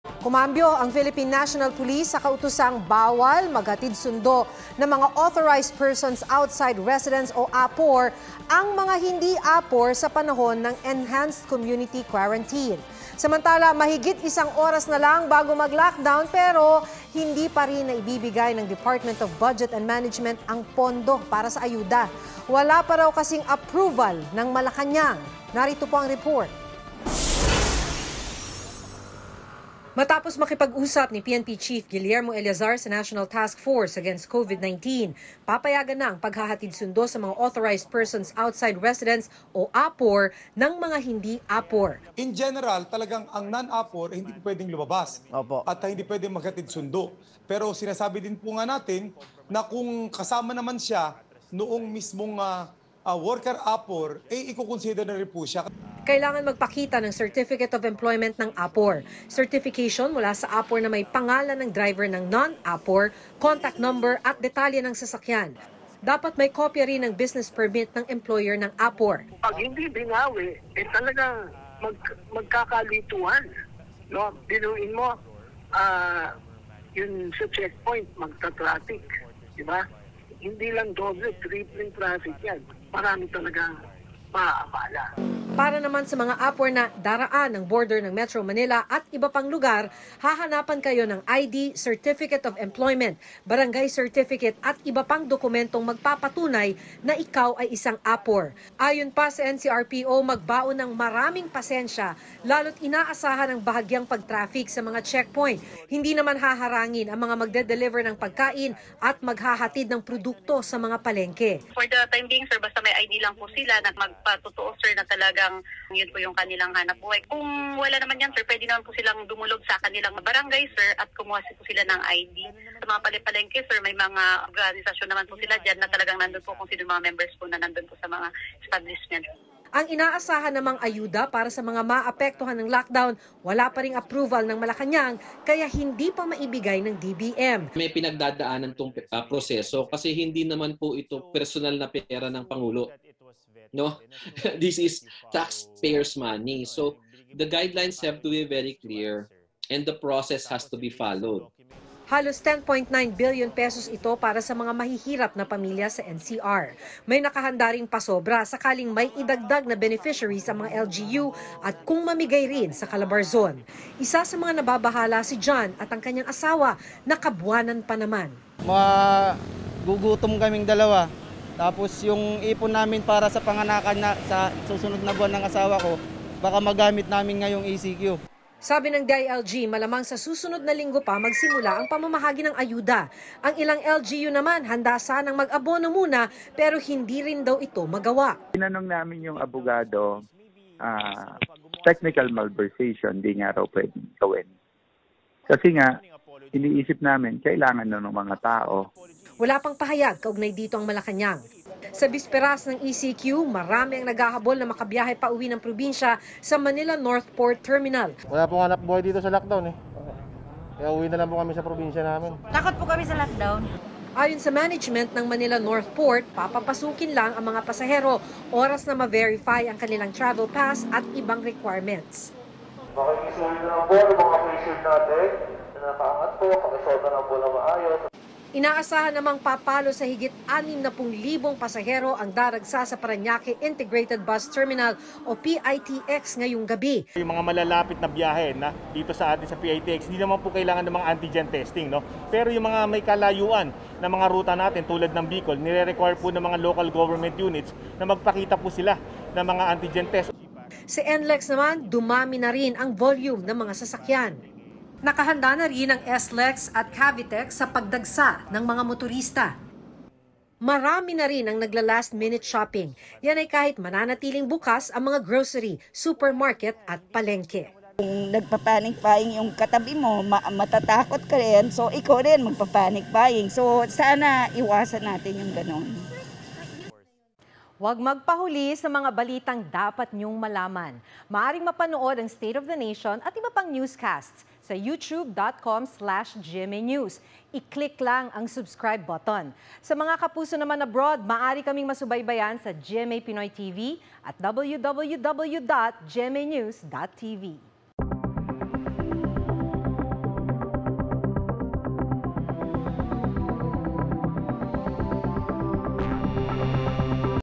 Non-APORs allowed to fetch essential workers during NCR ECQ —Eleazar | SONA
Essential workers will be allowed to be fetched by people who are not considered authorized persons outside residence, or non-APORs, when the enhanced community quarantine is enforced in Metro Manila, Philippine National Police chief Police General Guillermo Eleazar said Thursday.